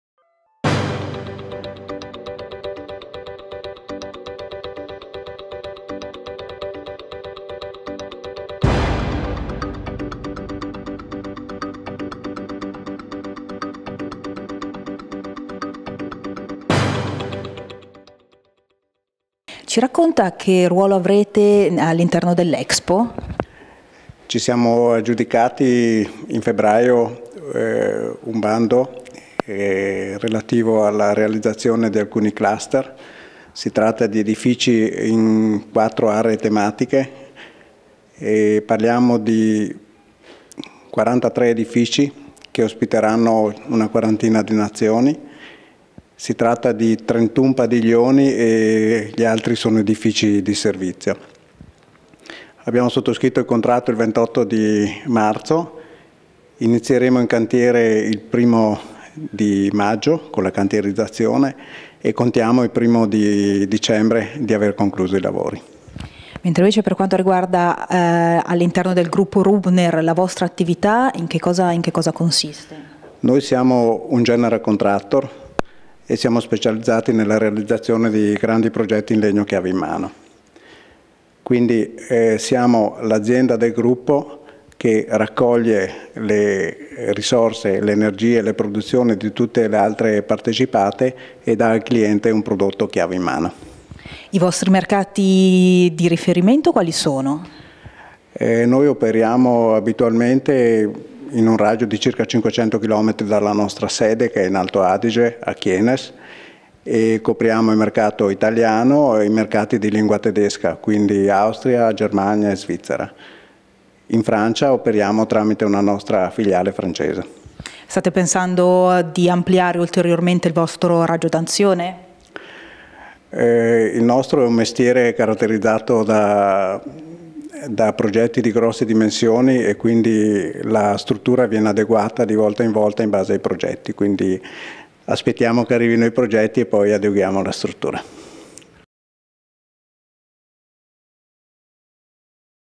Rubner ad Expo: intervista ad Alessandro Lacedelli
Otto mesi di tempo per realizzare quattro dei più importanti padiglioni cluster destinati a Expo Milano. Alessandro Lacedelli, amministratore delegato di Rubner Objektbau, racconta dettagli e tempistiche del progetto.